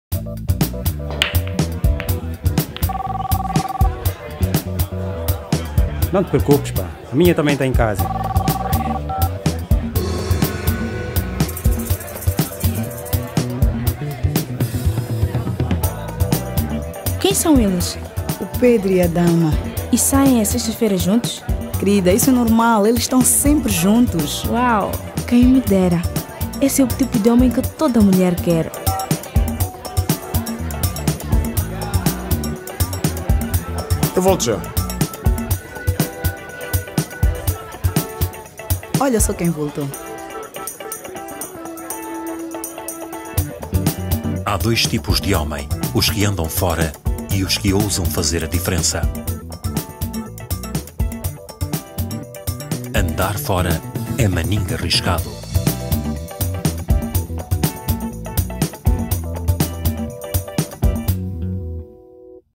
BAR
Andar fora é maningue arriscado.
Spot do Bar
Johns Hopkins University
Mahla Filmes Lda Production
Directed by Michele Mathison
Producers Mickey Fonseca and Pipas Forjaz
Shot and Edited by Pipas Forjaz